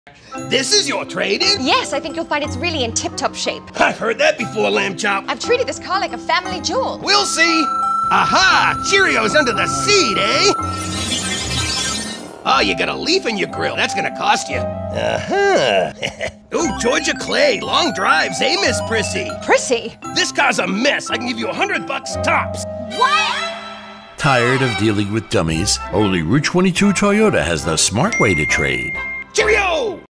TRADE IN
A couple of funny puppet spots I had fun shooting.  Tom Avitabile Director.